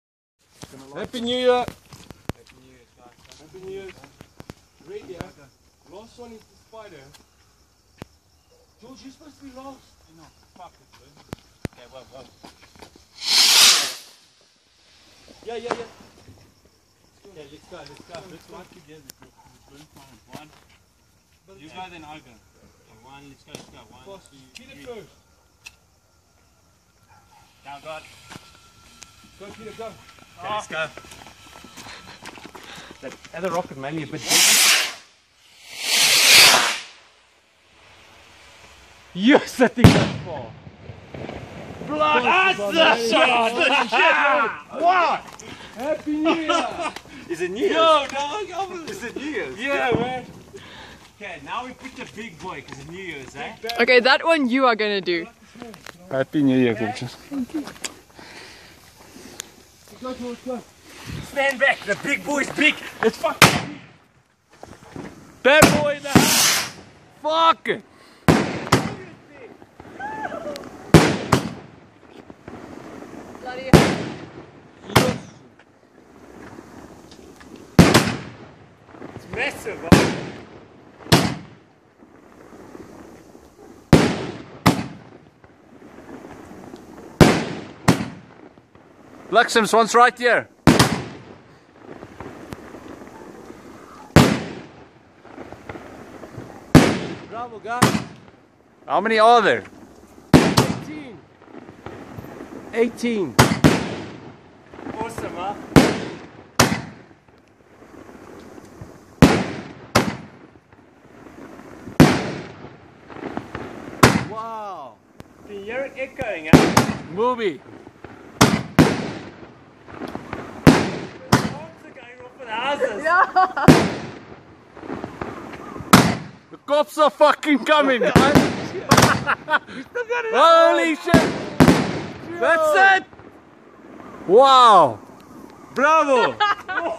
Happy New Year South Africa (fireworks Display)2014-2015
1x display she'll rocket 2x large rockets and 1x   18 shooter 'THE BOX' a.k.a "THE BIG BOY" 'More like a wow moment than fireworks but with power'

I would like to wish everyone a Happy and Prosperous New Year and Years ahead...